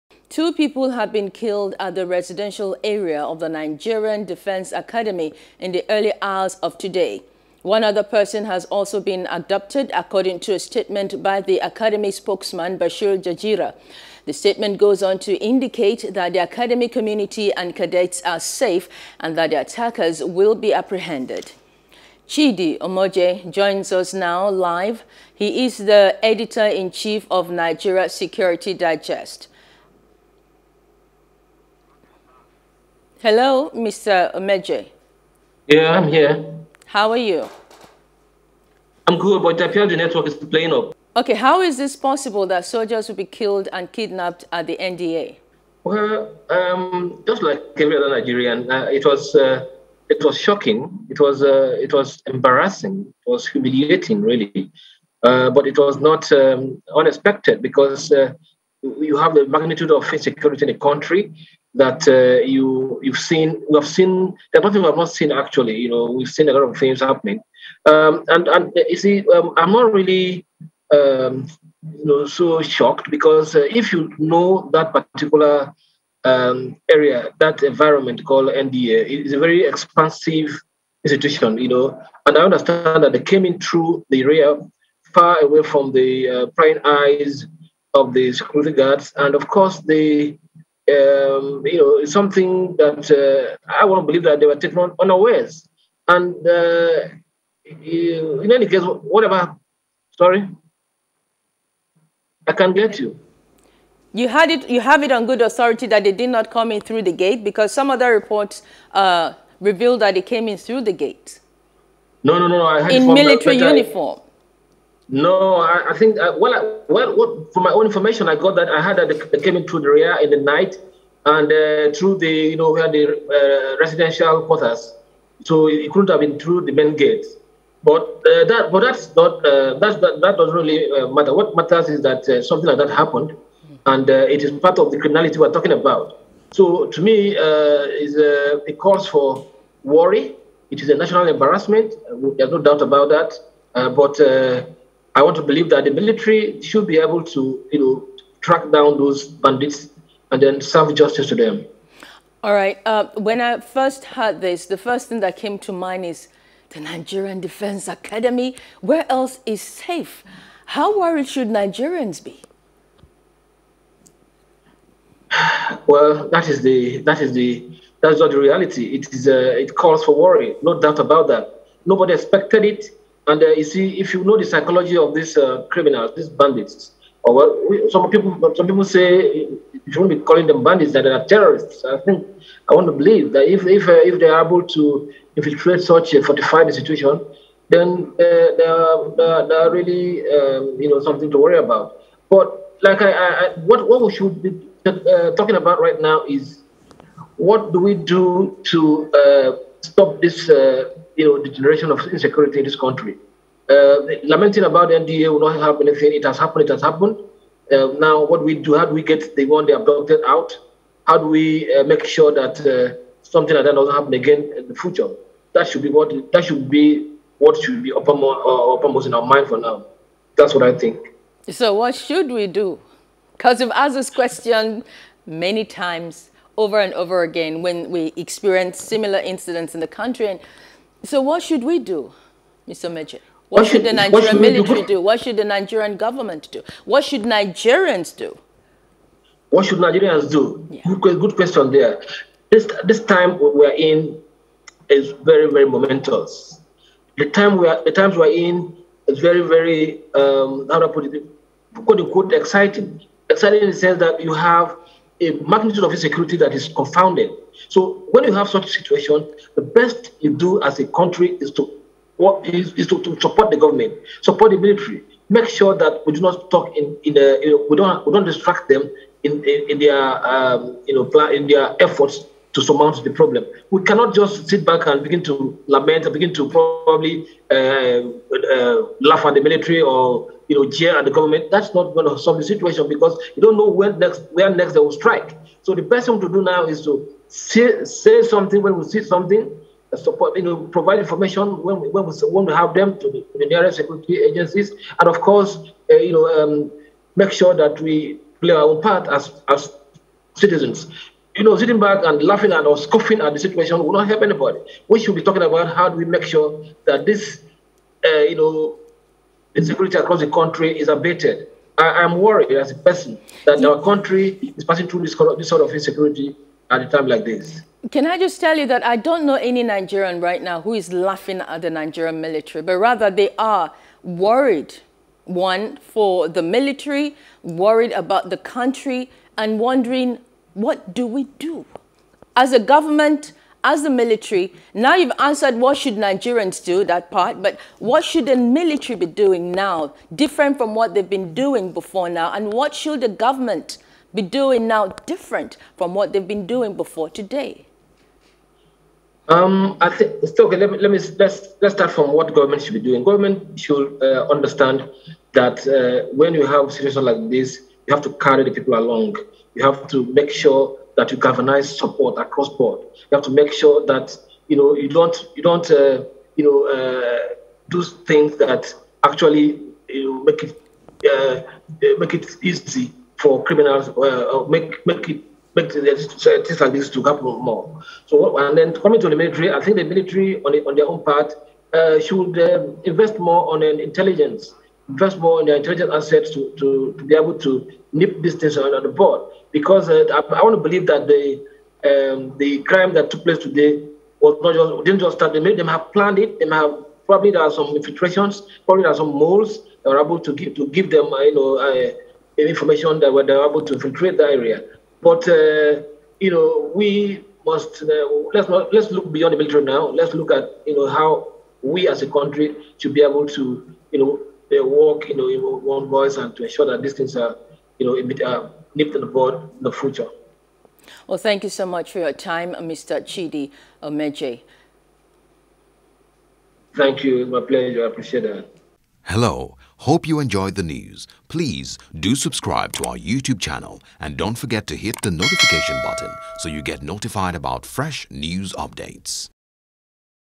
Security Breach: Gunmen Kill Two, Abduct One At Nigerian Defence Academy | NEWS
Two people have been killed at the residential area of the Nigerian Defence Academy in the early hours of yesterday.

Chidi Omeje, Editor-In-Chief, Nigeria Security joined us to discuss.